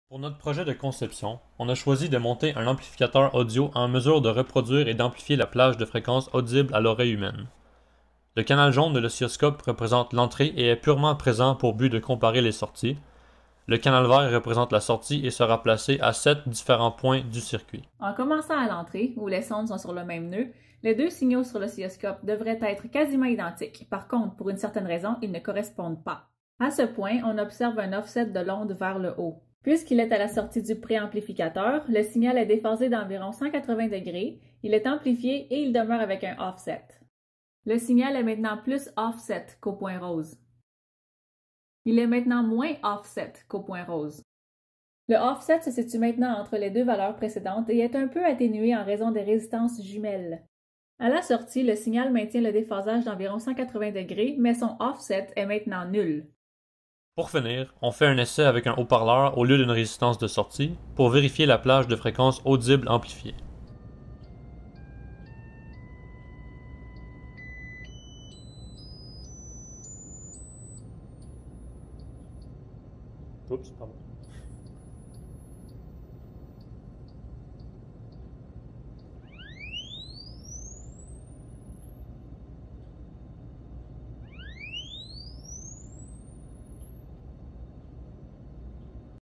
Projet Électronique 2: amplificateur audio
Projet du cours Électronique 2 à l'université du Québec en Outaouais.
Marianne Pagé-Potvin et David Azzie: amplificateur audio.